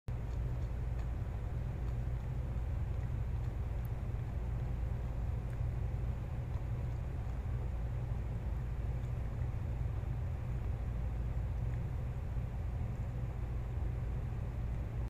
Catnip Video #2- transition from play to licking
I decided to get the cat high on catnip while I'm in isolation and video it in 15 second shorts.